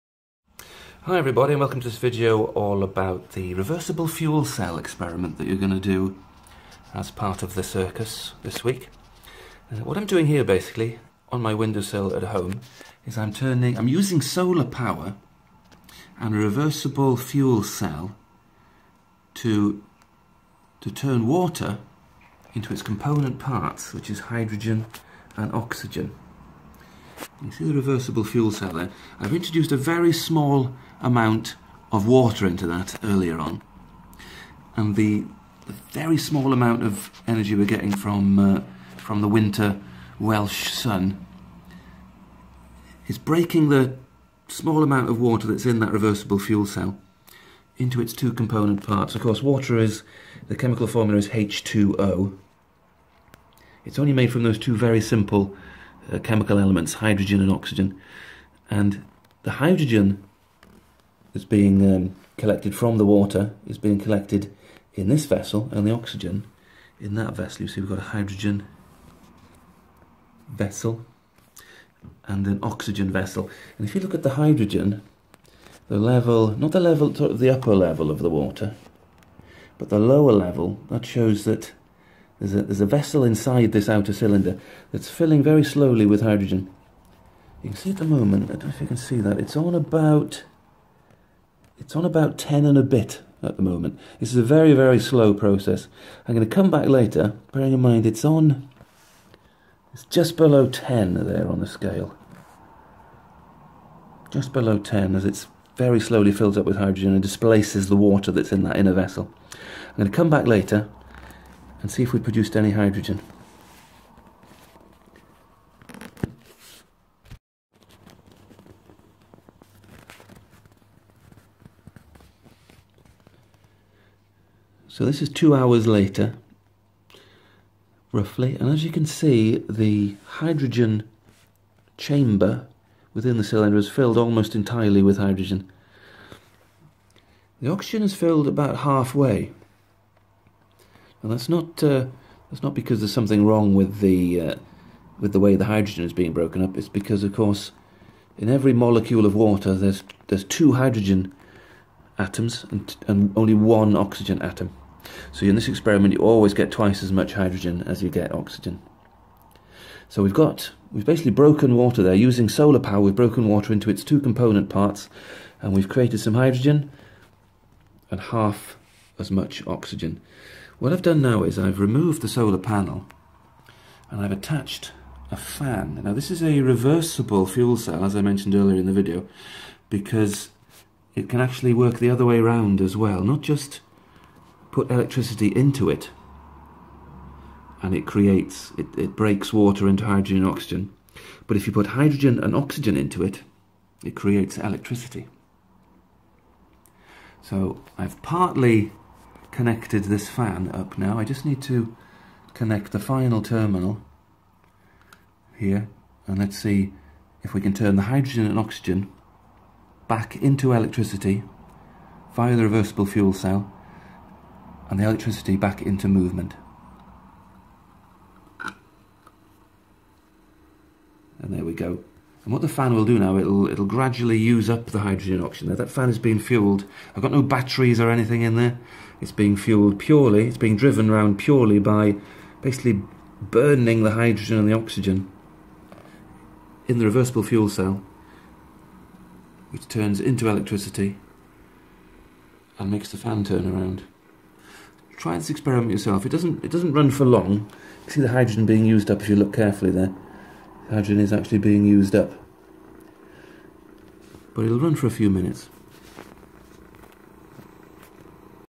Reversible fuel cell
Using the power of the sun to break water into its component parts, hydrogen and oxygen, then reversing the process to produce electricity.